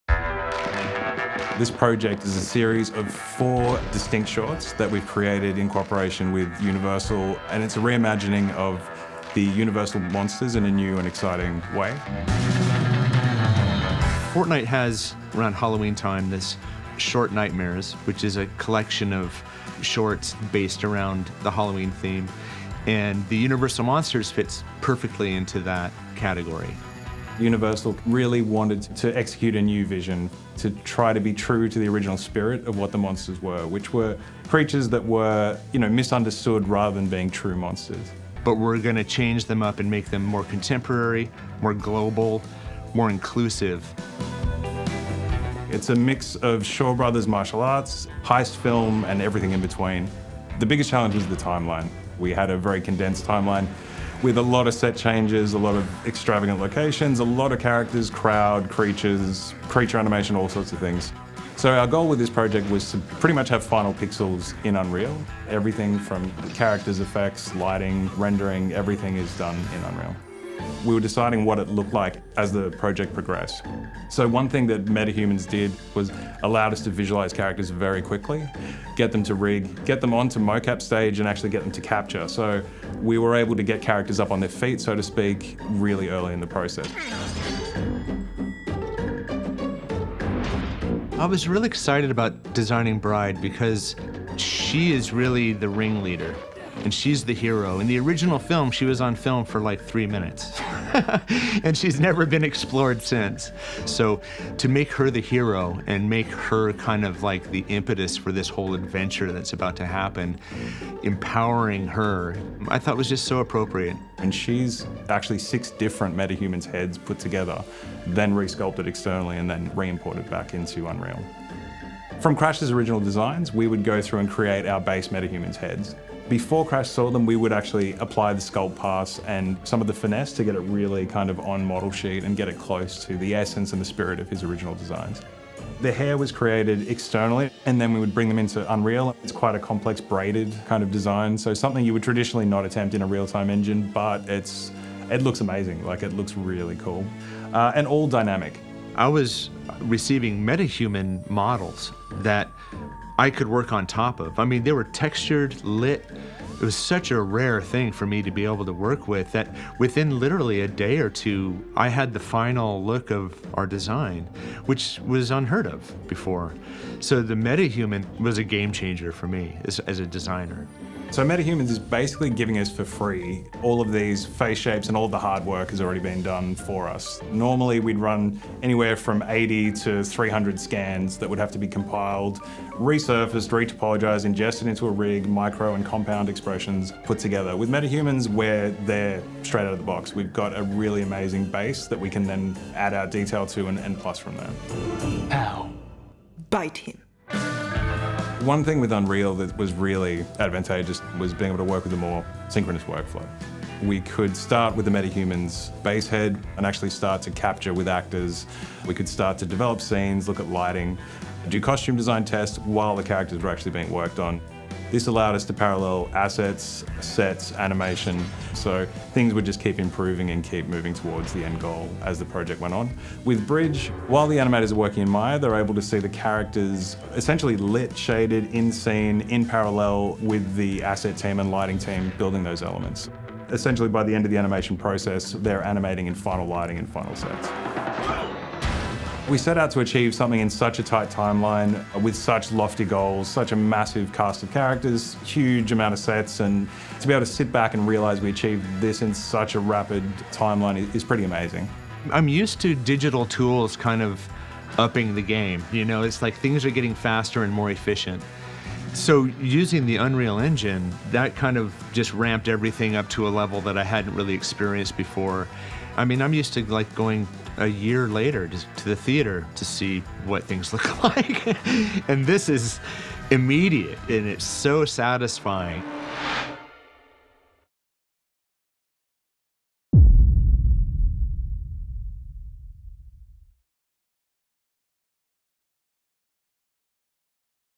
Making Universal Monsters with Real-Time Animation | Spotlight | Unreal Engine
Hear how Plastic Wax brought Dracula and The Bride of Frankenstein to Fortnite using MetaHuman Creator and Unreal Engine.